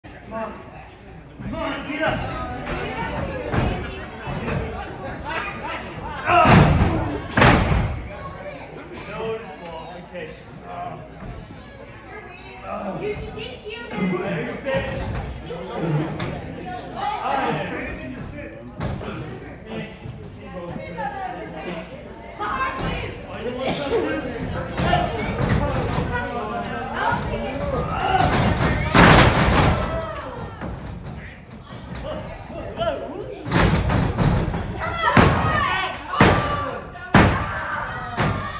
TCW 10/17/24 Triple Threat Clip
Kreigh Folsom hitting a sick move during the triple threat hardcore halloween match in Jeffersonville, indiana.Indiana.